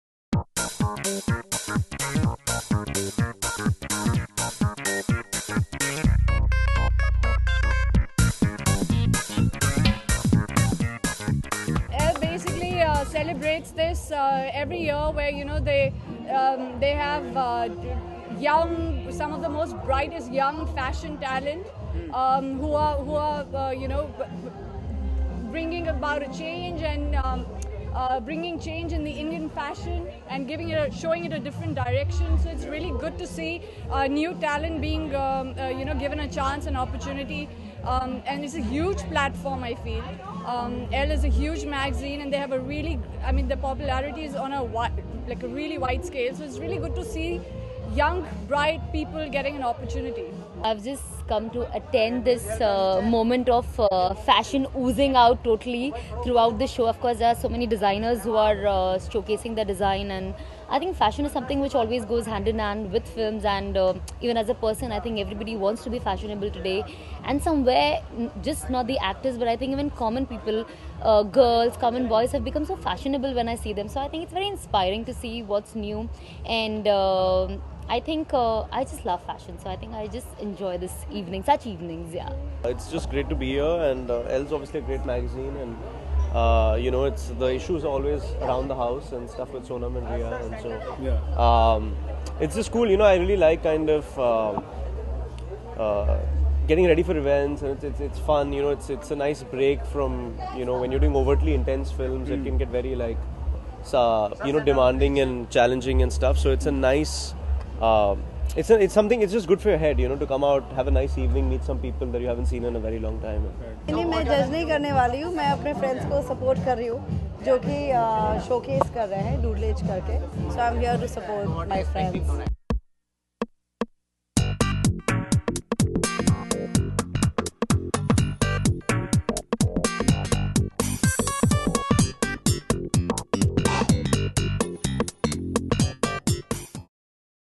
Latest Bollywood News - Vaani Kapoor At Elle India Fashion Show - Bollywood Gossip 2016
Watch this exclusive interview of Vaani Kapoor.